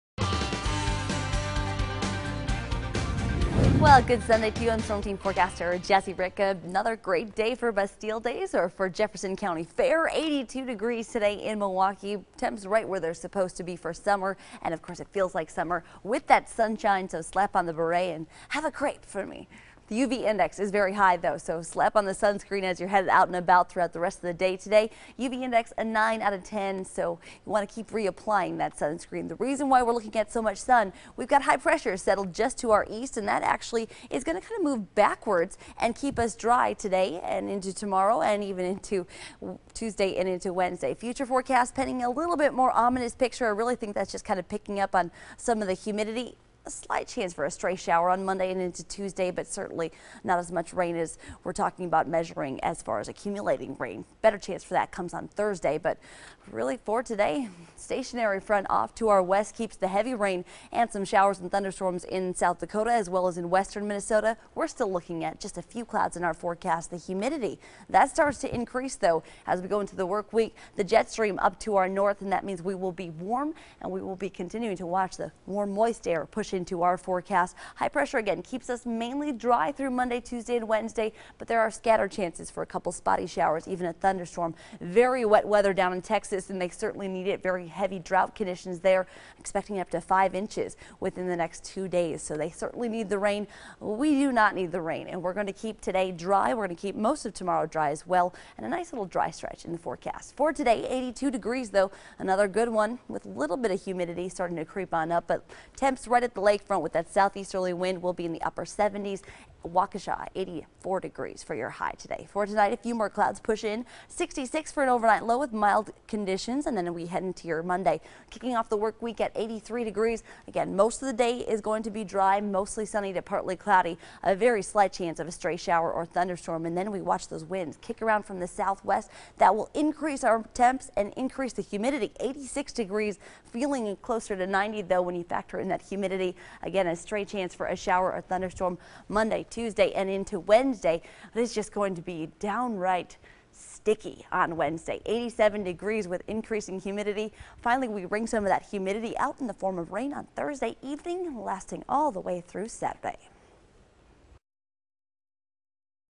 Storm Team 4: Another nice summer day ahead!
The dry stretch in Southeastern WI continues with mostly sunny skies and low 80's expected.